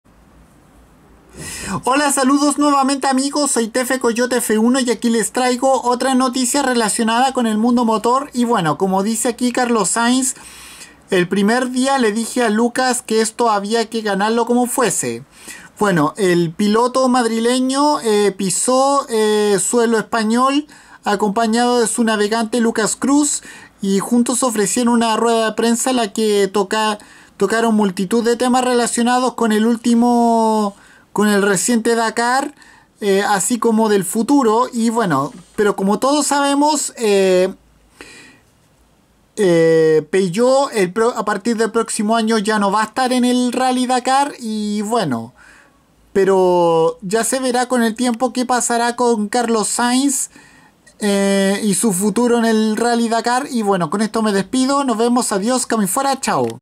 Carlos Sainz le asegura a Lucas que el Dakar lo podrían haber conseguido
El piloto madrileño llegó a España y hablaron de distintos temas relacionados con la reciente edición del Rally Dakar 2018 y de lo que se espera para el futuro